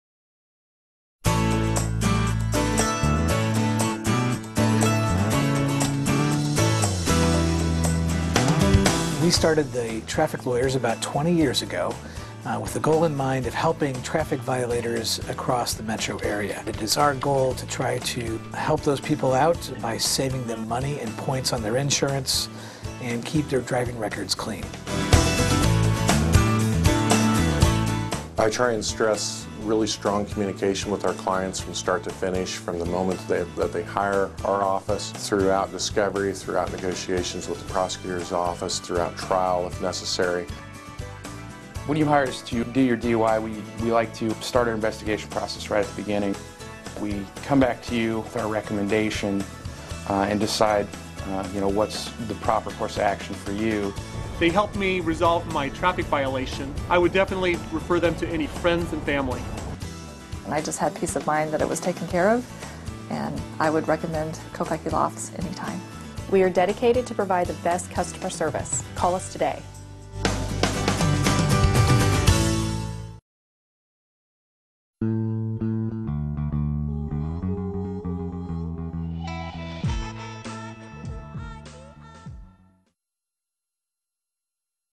Overland Park, KS | The Traffic Lawyers of Kopecky Law, P.A.
Got a ticket? At Traffic Lawyers of Kopecky Law, P.A. in Overland Park, Kansas, they offer aggressive legal representation in areas such as DUIs and DWIs in Kansas, Missouri, and Iowa, minor in possession (MIP), driving while suspended (DWS) or revoked (DWR), drug charges and possession, commercial driver license (CDL), probation violations or diversion revocations, careless and reckless driving, speeding tickets and other traffic violations.